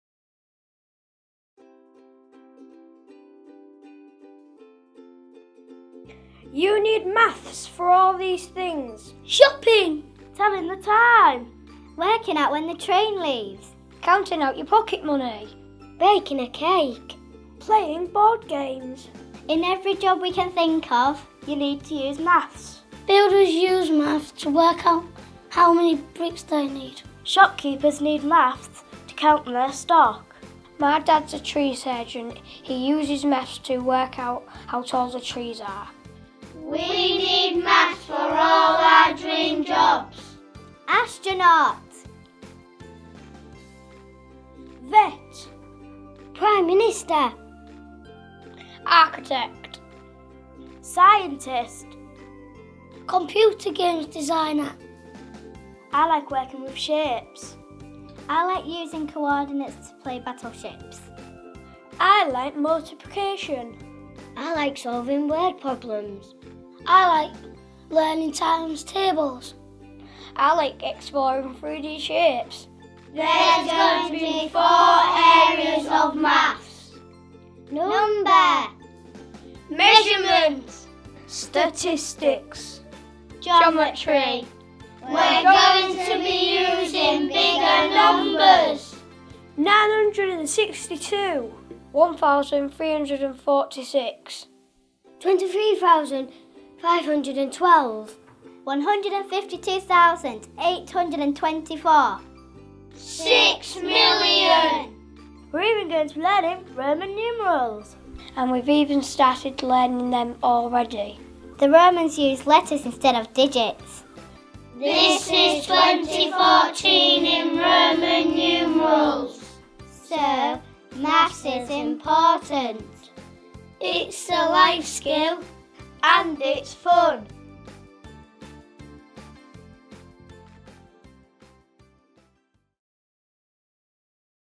New Primary Maths Curriculum 2014 - Swinemoor Primary School
The new primary maths curriculum begins in September 2014. We speak with our pupils about the various uses for maths and what they enjoy.